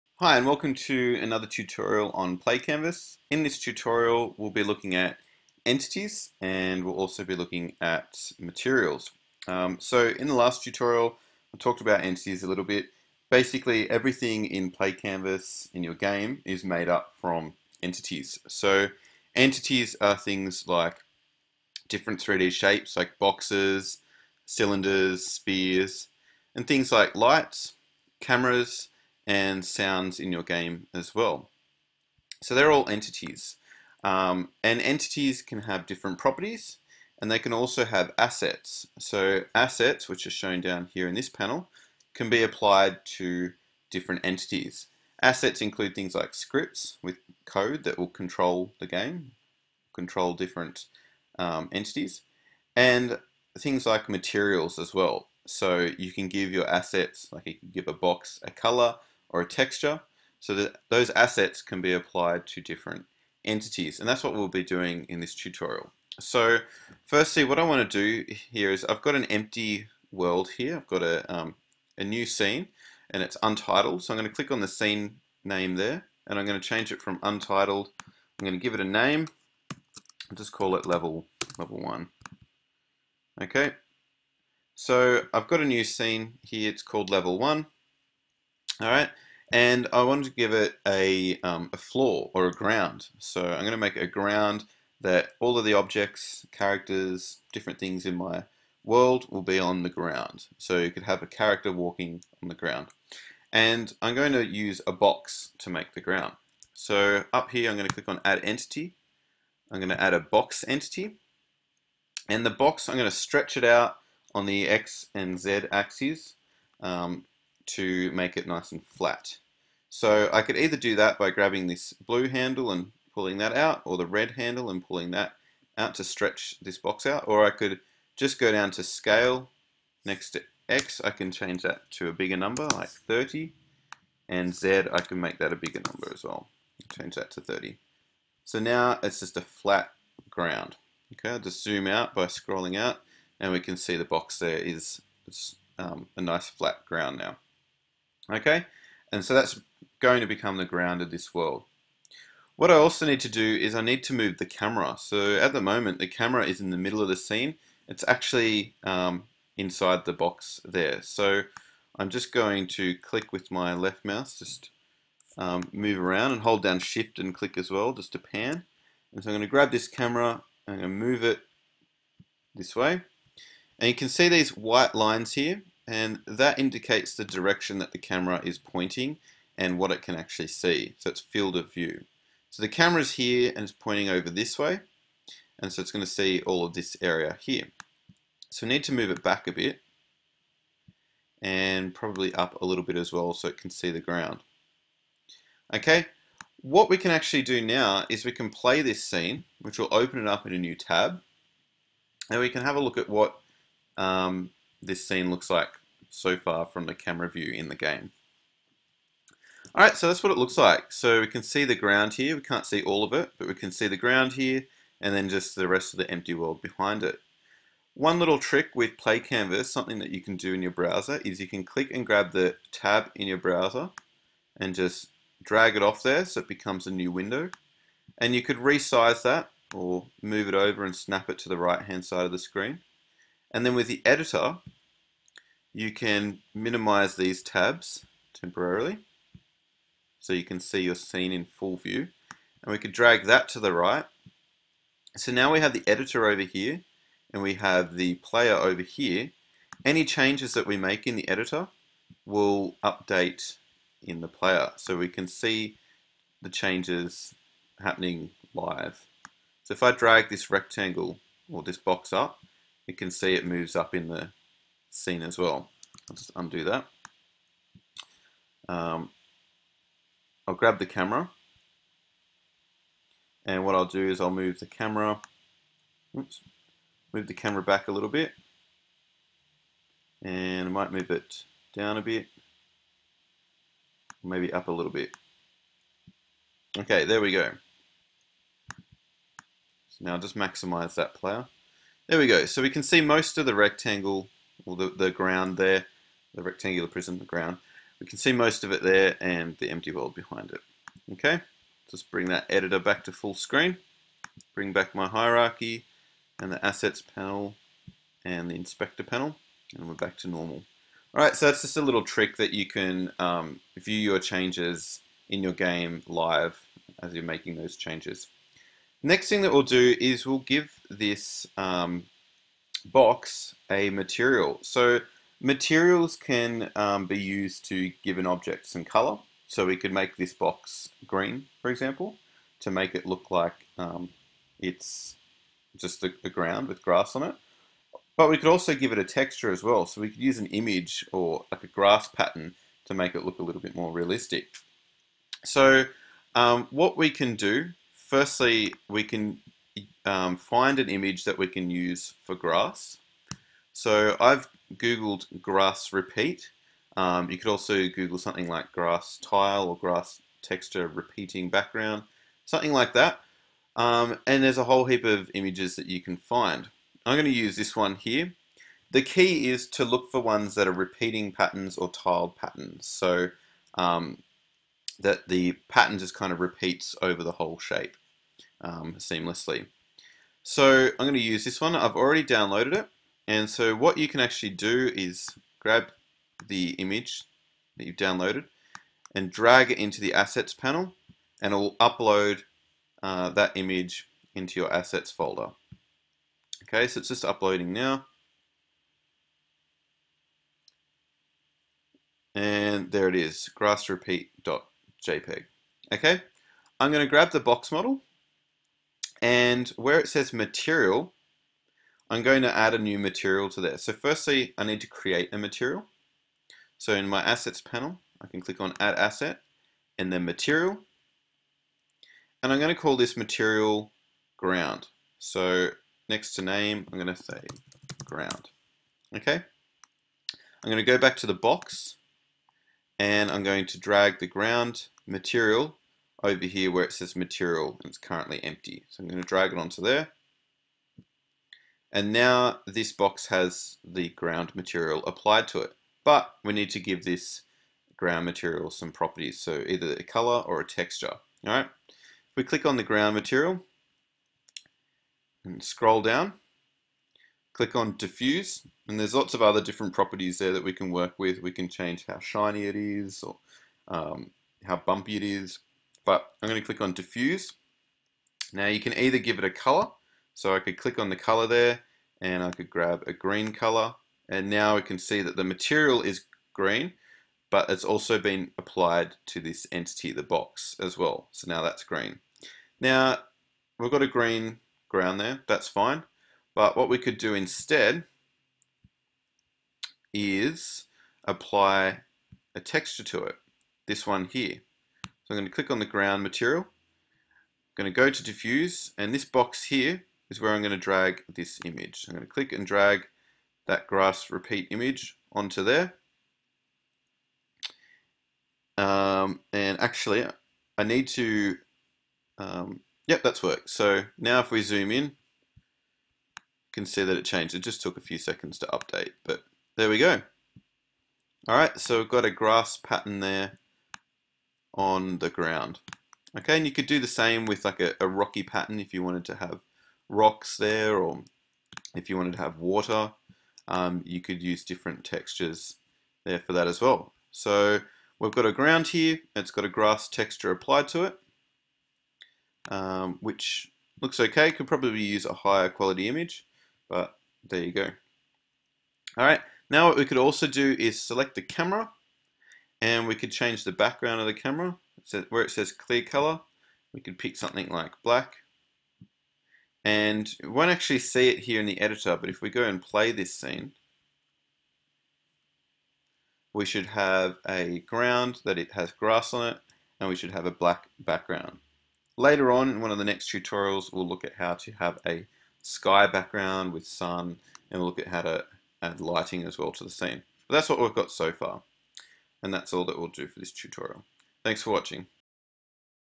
PlayCanvas Tutorial 2 - Entities and materials
In this tutorial you will learn more about entities and how to modify a Box entity to make a ground or floor for your game’s scenes. You will also learn how to use materials to give 3D objects some colour and apply detailed patterns and textures using assets.